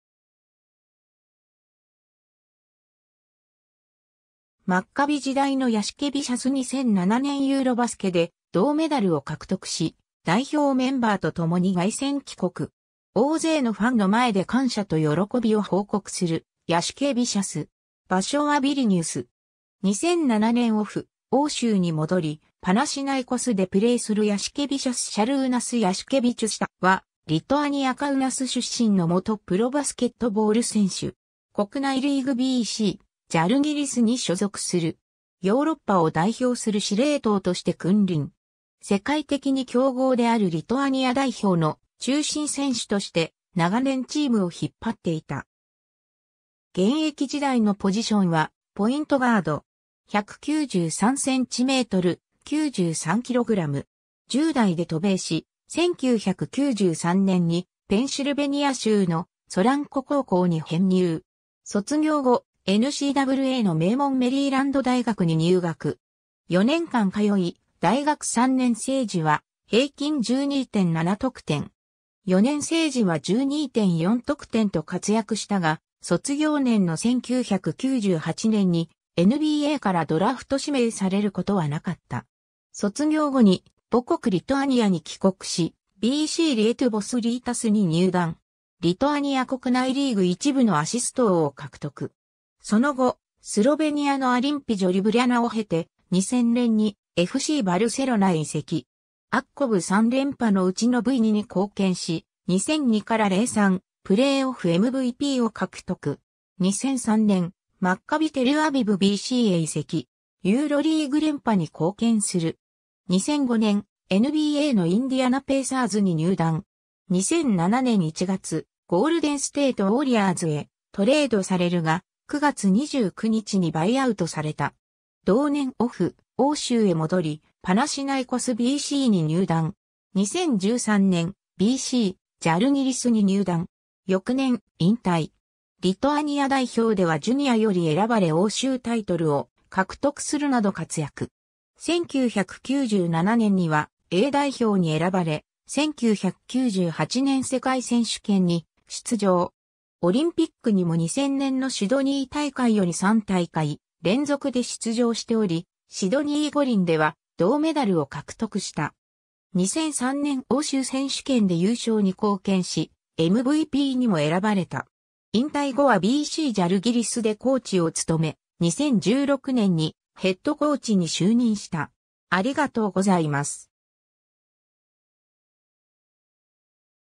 シャルーナス・ヤシケヴィチュス
KKユニオン・オリンピアの選手
FCバルセロナ_(バスケットボール)の選手
マッカビ・テルアビブBCの選手
マッカビ時代のヤシケヴィシャス 2007年ユーロバスケで銅メダルを獲得し、代表メンバーと共に凱旋帰国。
大勢のファンの前で感謝と喜びを報告するヤシケヴィシャス。
場所はビリニュス。
 2007年オフ、欧州に戻りパナシナイコスでプレイするヤシケヴィシャス(19番) シャルーナス・ヤシケヴィチュス（Šarūnas Jasikevičius 発音、1976年3月5日 - ）は、リトアニア・カウナス出身の元プロバスケットボール選手。
国内リーグBCジャルギリスに所属する。
ヨーロッパを代表する司令塔として君臨。
世界的に強豪であるリトアニア代表の中心選手として、長年チームを引っ張っていた。
現役時代のポジションはポイントガード。
193cm、93kg。
 10代で渡米し、1993年にペンシルベニア州のソランコ高校に編入。
卒業後、NCAAの名門メリーランド大学に入学。
4年間通い、大学3年生時は平均12.7得点。
4年生時は12.4得点と活躍したが、卒業年の1998年にNBAからドラフト指名される事はなかった。
 卒業後に母国リトアニアに帰国し、BCリエトゥヴォス・リータスに入団。
リトアニア国内リーグ1部のアシスト王を獲得。
 その後、スロベニアのOlimpija Ljubljanaを経て、2000年にFCバルセロナへ移籍。
ACB3連覇のうちのV2に貢献し、2002-03プレーオフMVPを獲得。
 2003年、マッカビ・テルアビブBCへ移籍。
ユーロリーグ連覇に貢献する。
 2005年、NBAのインディアナ・ペイサーズに入団。
 2007年1月、ゴールデンステート・ウォリアーズへトレードされるが、9月29日にバイアウトされた。
 同年オフ、欧州へ戻り、パナシナイコスBCに入団。
 2013年、BCジャルギリスに入団。
翌年、引退。
 リトアニア代表ではジュニアより選ばれ欧州タイトルを獲得するなど活躍。
 1997年にはA代表に選ばれ、1998年世界選手権に出場。
 オリンピックにも2000年のシドニー大会より3大会連続で出場しており、シドニー五輪では銅メダルを獲得した。
 2003年欧州選手権で優勝に貢献し、MVPにも選ばれた。
 引退後はBCジャルギリスでコーチを務め、2016年にヘッドコーチに就任した。